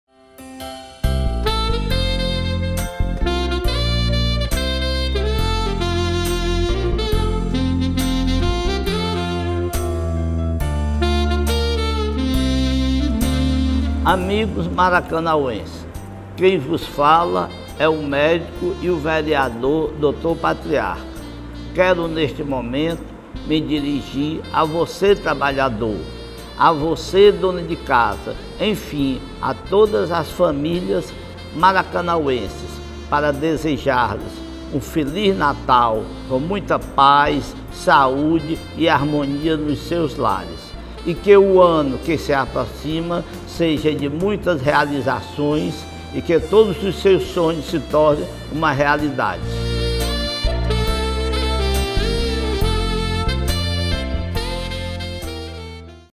O vereador José Patriarca Neto (Dr. Patriarca)
O vereador de Maracanaú, José Patriarca Neto (Dr. Patriarca), deseja a todos um Feliz Natal, um próspero 2015 e Boas Festas.